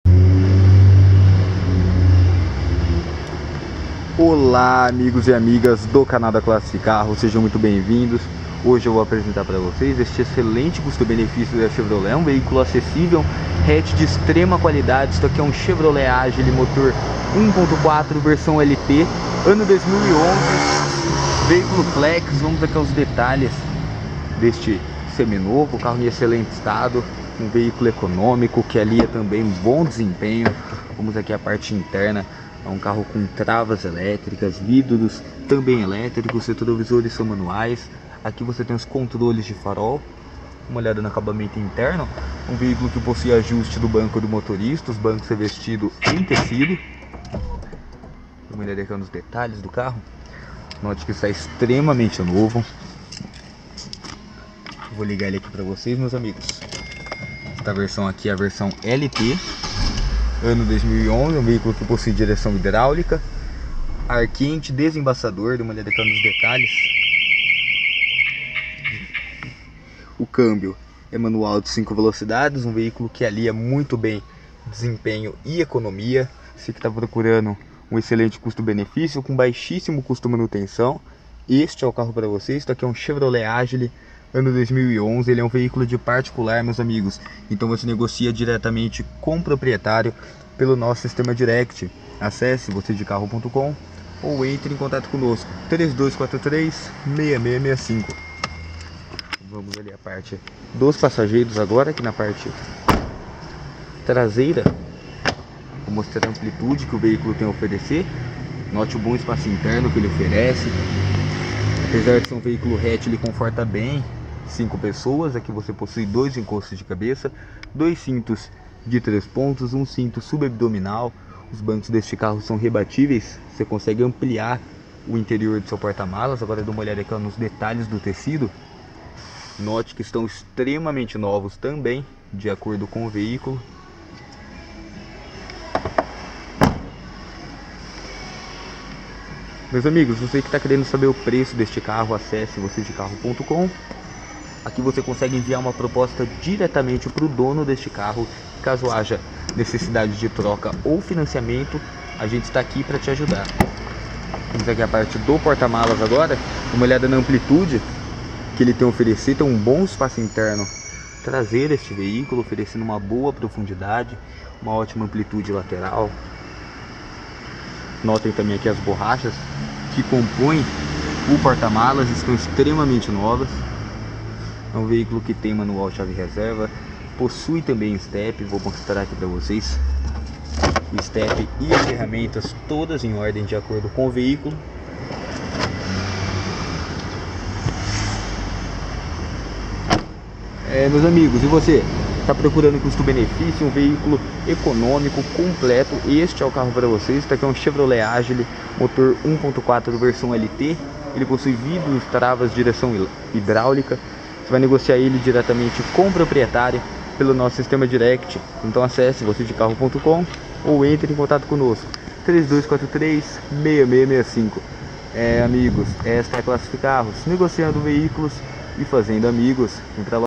Chevrolet Agile LT 1.4 8v (Flex) - 2011
NOVÍSSIMO, C/MANUAL E CHAVE RESERVA E REVISÕES CARIMBADAS NO MANUAL. O AGILE É SINÔNIMO DE ÓTIMO CUSTOxBENEFÍCIO, BAIXO CUSTO MANUTENÇÃO, ECONOMIA E ACESSIBILIDADE PARA SEU DIA-DIA, LAZER E USO COMERCIAL. VERSÃO LT TRÁS DIREÇÃO, VIDROS E TRAVAS ELTC.